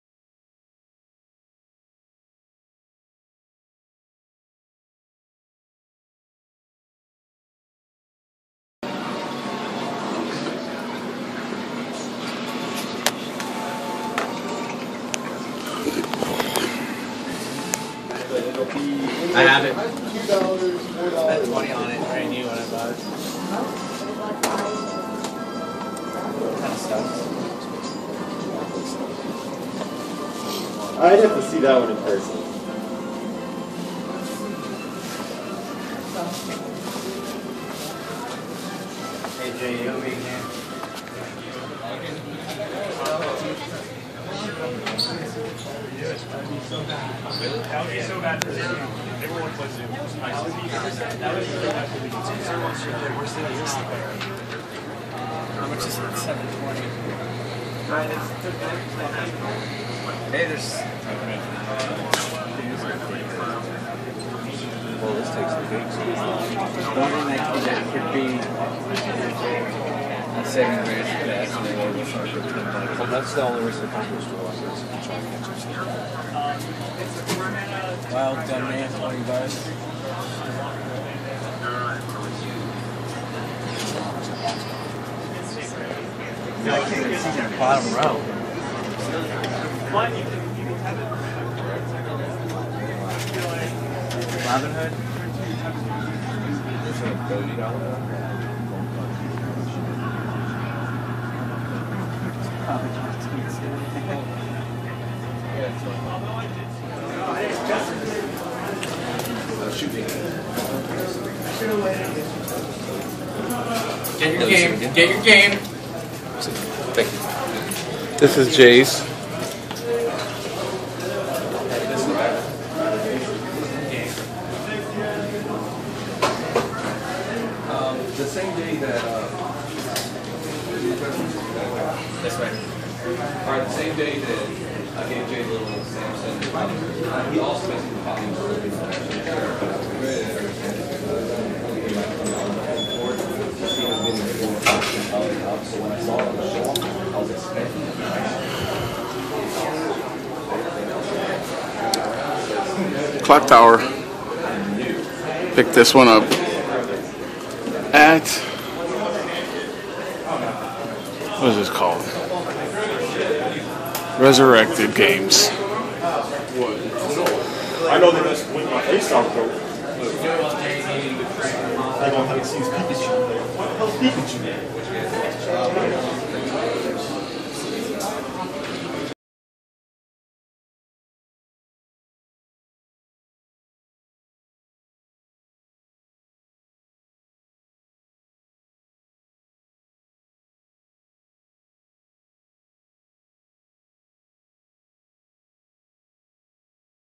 Okchief420 Video Game Hunting EP. 14 TheGameChasers In Austin, TX
The GameChasers and 8BitEric came and spent the weekend with me and we had a blast Game Chasing and playing games in Austin, TX. A behind the scenes if you will of a GameChasers episode.
Thanks to Gamers Exchange in Austin, TX for letting us Film
Thanks to resurrected games in Austin, TX for letting me film
Tell them okchief420 sent you!!